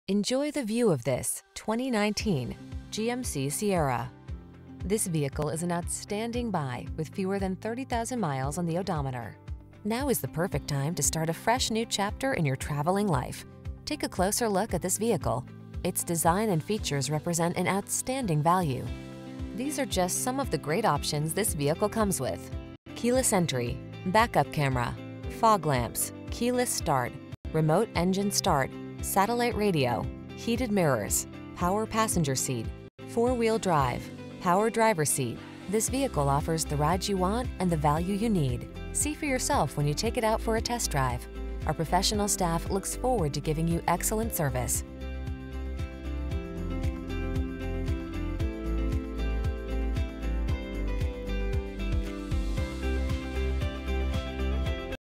2019 GMC Sierra 1500 Jasper, Cartersville, Alpharetta, Dawsonville, Canton, North Georgia P10568
Quicksilver Metallic Used 2019 GMC Sierra 1500 available in Canton, Georgia at CHevrolet of Canton. Servicing the Jasper, Cartersville, Alpharetta, Dawsonville, North Georgia area.
2019 GMC Sierra 1500 SLT - Stock#: P10568 - VIN#: 3GTU9DED7KG101707

Quicksilver Metallic 2019 GMC Sierra 1500 SLT 4WD 8-Speed Automatic EcoTec3 5.3L V8 8-Speed Automatic, 4WD, Jet Black Leather.Recent Arrival! Odometer is 9213 miles below market average!